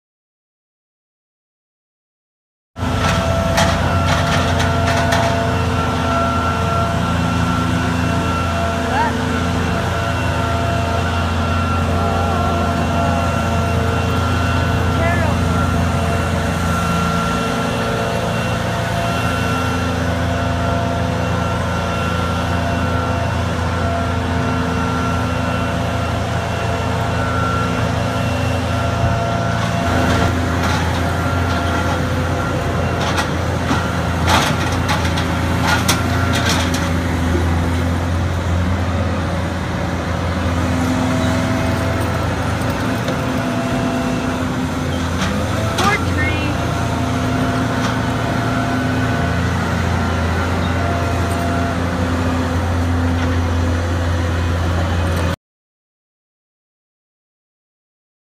Oak tree near Sylvester's Burgers is cut down
A 100-year-old coast live oak tree near Sylvester's Burgers in Atascadero was chopped down Saturday, Jan. 10, 2015. | Video by Joe Tarica